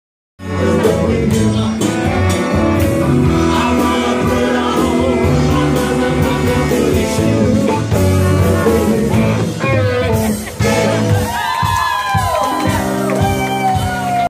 PG BLACK WATER BLUES BAND APRIL 2015(1)
Video of Blackwater blues band at 2001 club april 2015 taken by sheila world of photography